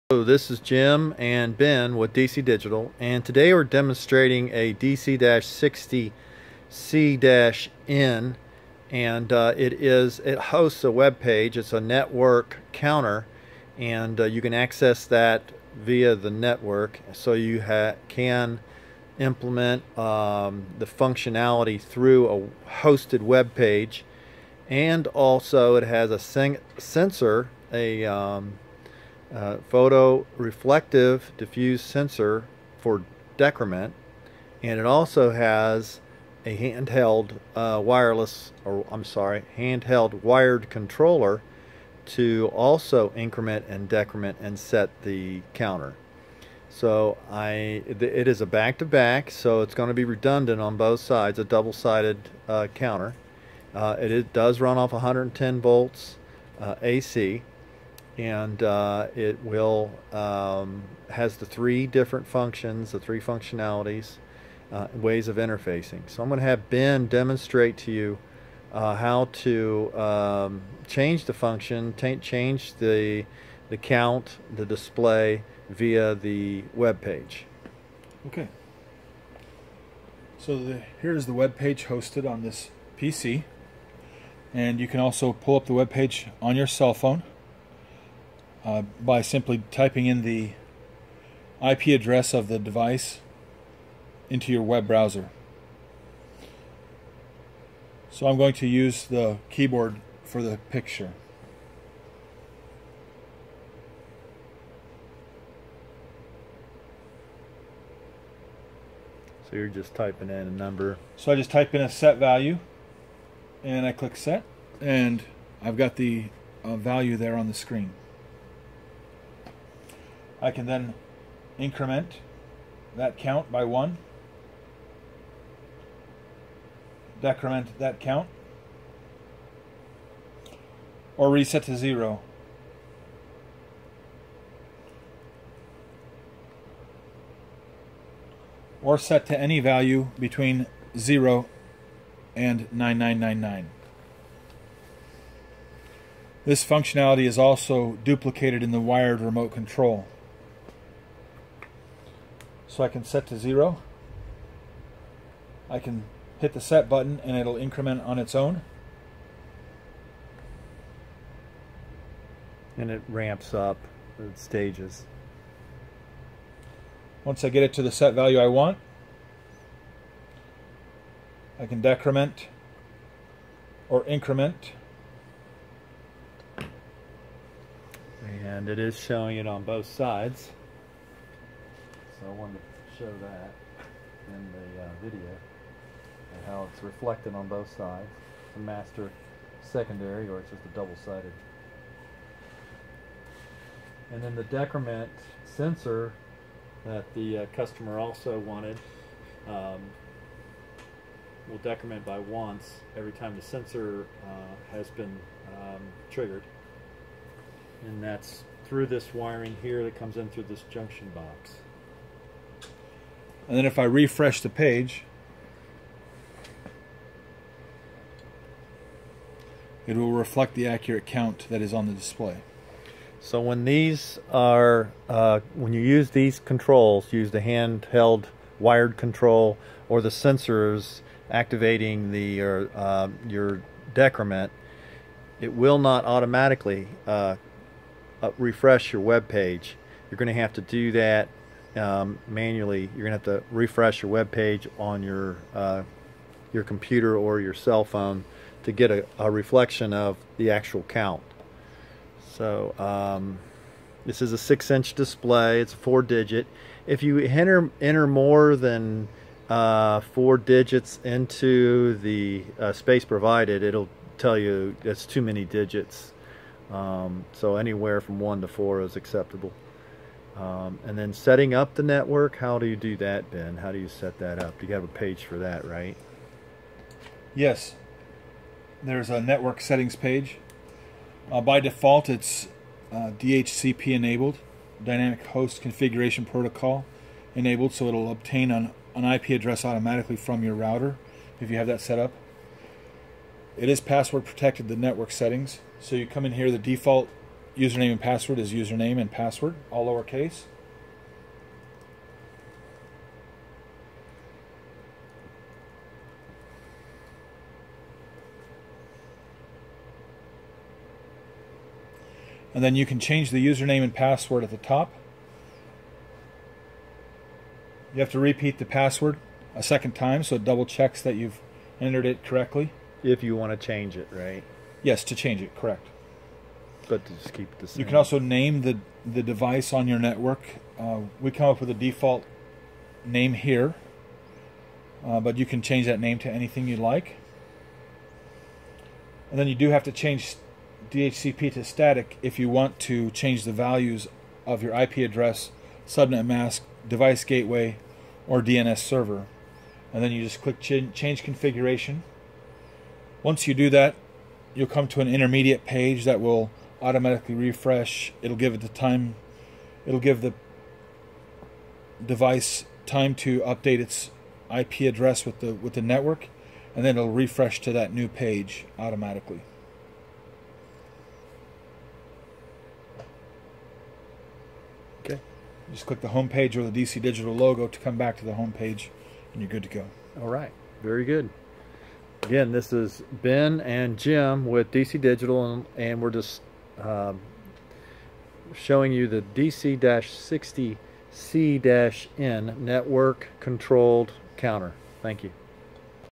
DC-60C-N (Network and Local Push Button Controlled Digital Counter)
Network Web Page Controlled Counter with wired controller and decrement sensor. Control the counter through your network; Set the count, increment the count, decrement the count from the computer web page hosted on the counter. Also locally change the count with the handheld wired controller.